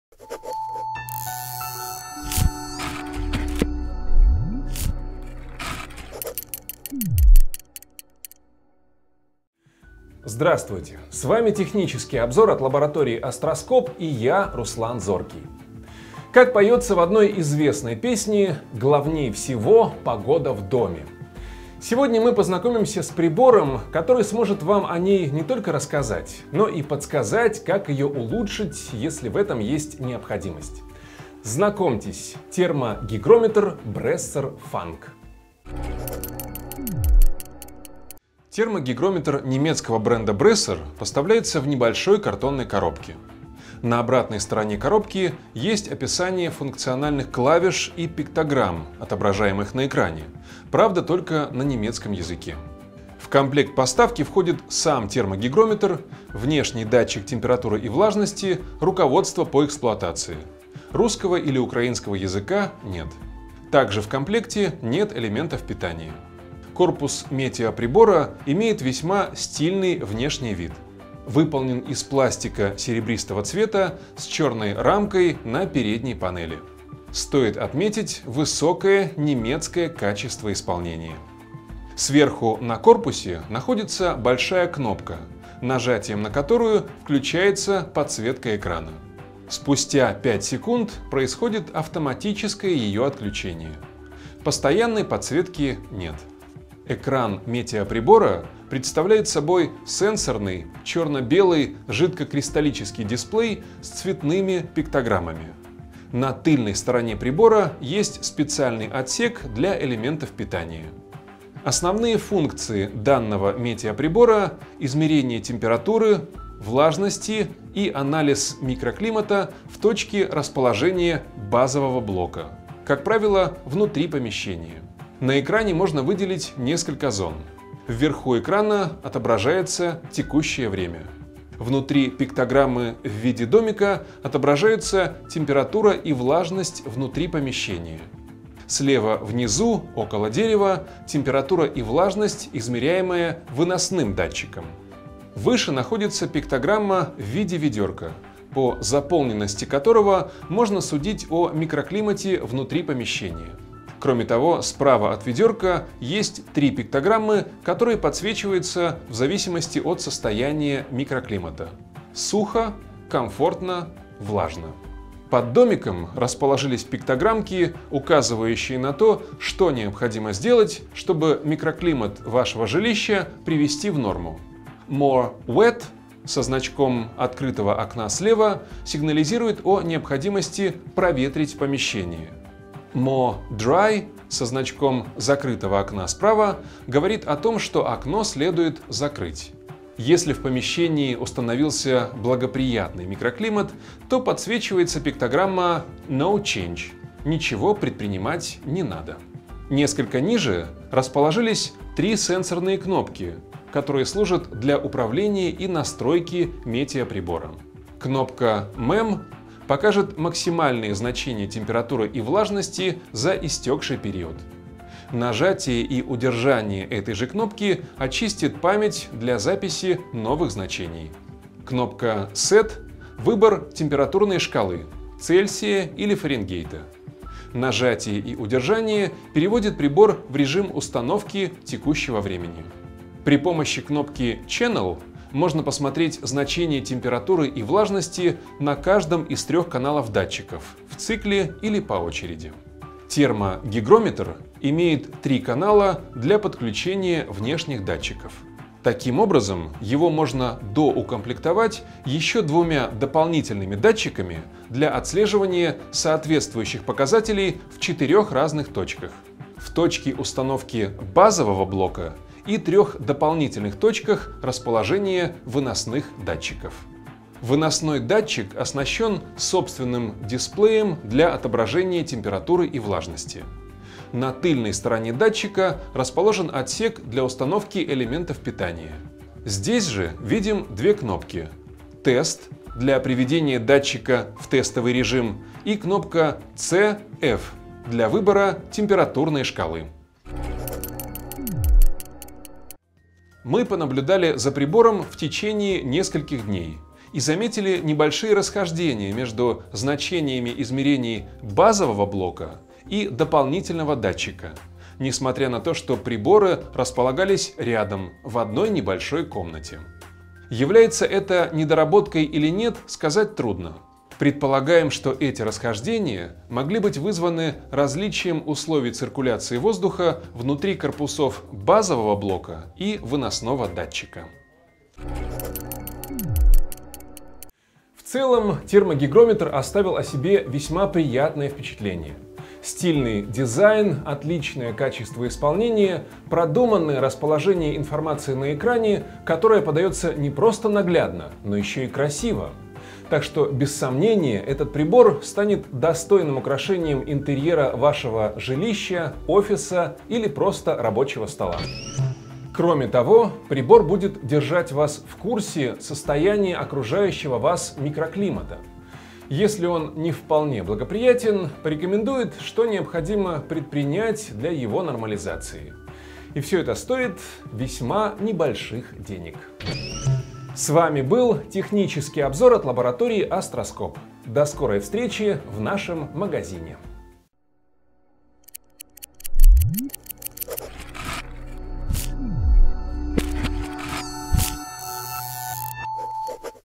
Обзор метеостанции Bresser Funk
Современный термометр-гигрометр Bresser Funk (Touchscreen) способен анализировать показатели температуры и влажности воздуха внутри и снаружи помещения и на основании полученного результата сигнализировать о необходимости проветривания помещения для достижения оптимального уровня комфорта внутри дома или офиса.